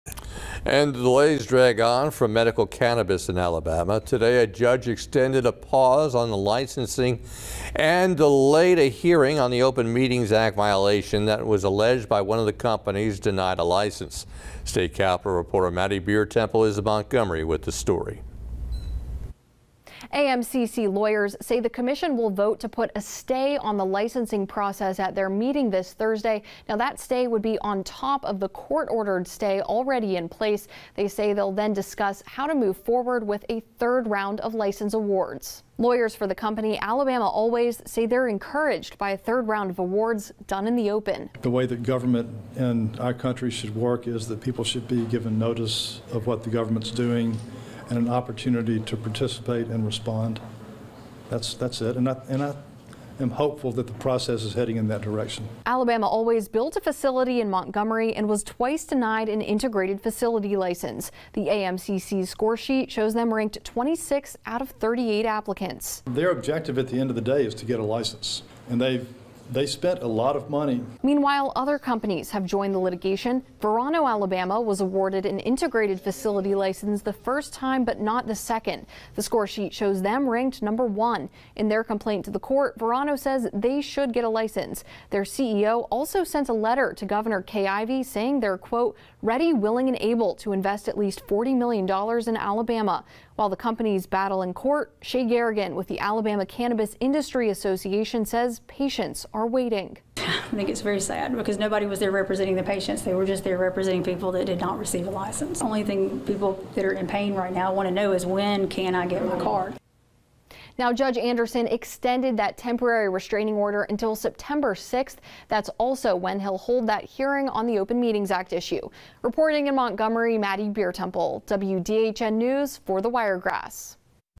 Judge continues legal troubles for Alabama Medical Cannabis
This issue will have to be decided later on in court.